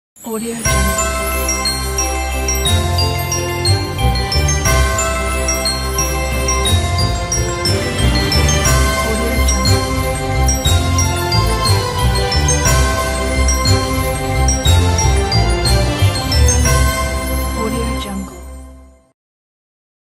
Christmas Tree Photos Opener | Apple Motion Templates
Compatible with Apple Motion 5.3.2 and higher versions.
Compatible with FCPX.
Everything is Customizable.
Have 21 Video/Image Dropzones.
Have 2 Editable Text.
No Plugins Needed.
Tutorial included for customizing the project.
All the Images used are from for Preview Purpose Only. Not Included in the Project.